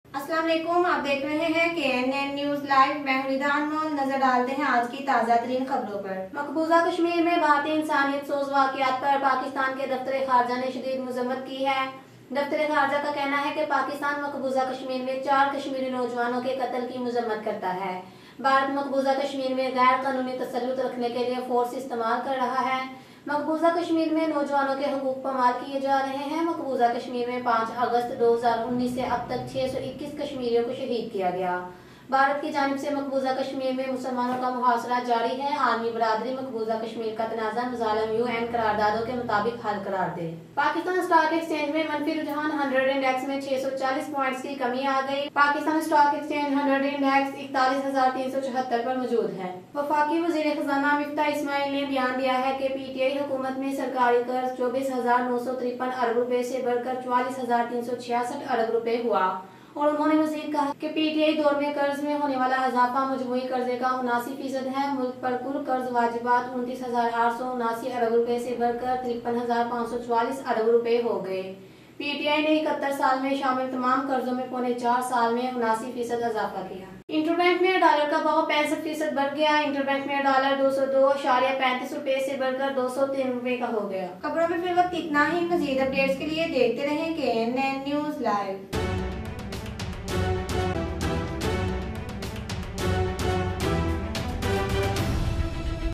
Maqboza Kashmir Mai Bharat Muzalim Par Pakistani Daftar e Kharja Ki Muzammat#KNN NEWS LIVE 13-06-22
Maqboza Kashmir Mai Bharat Muzalim Par Pakistani Daftar e Kharja Ki Muzammat
Pakistan Stock Exchange Mai Manfi Rujhan
Miftah Ismail Ky PTI Pr Elzamat
Inter Bank Mai Dollar 65 Paisy Barh Gya